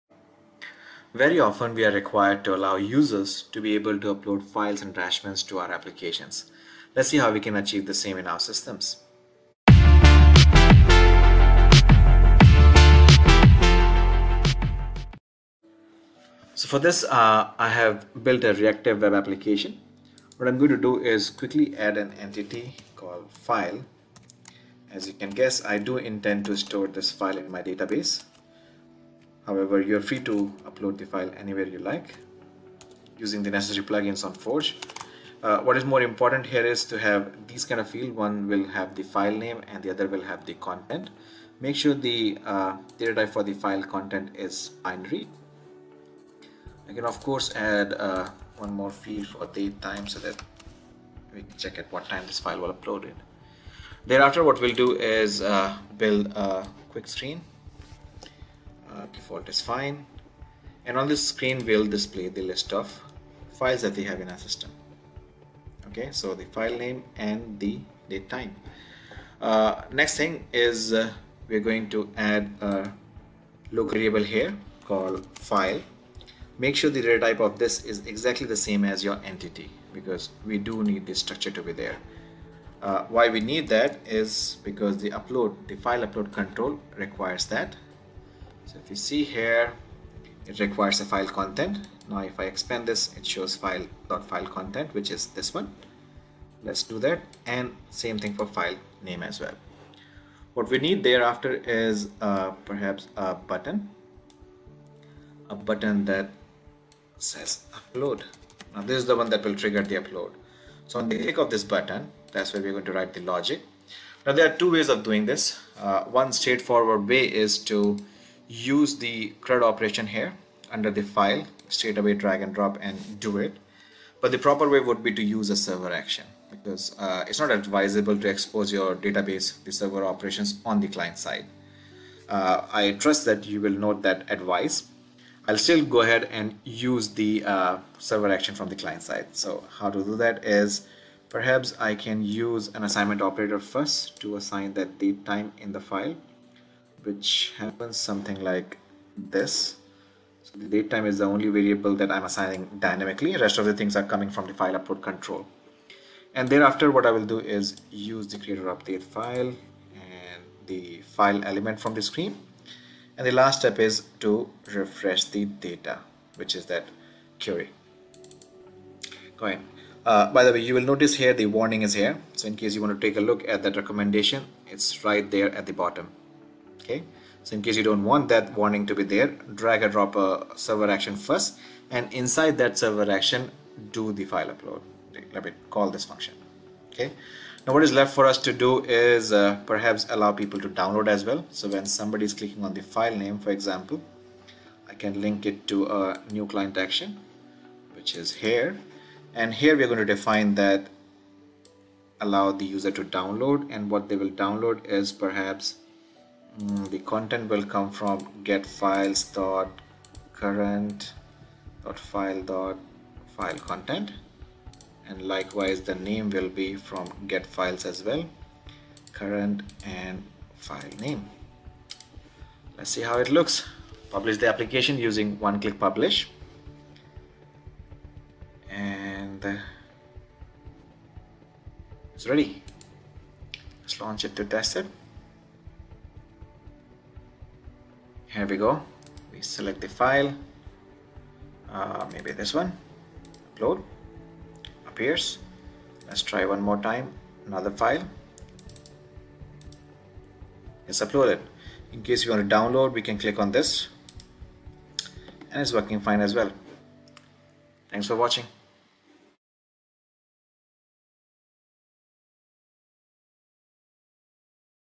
How to use file upload widget in OutSystems?
Easily add a file upload widget to your screens so users can upload files and attachments to your applications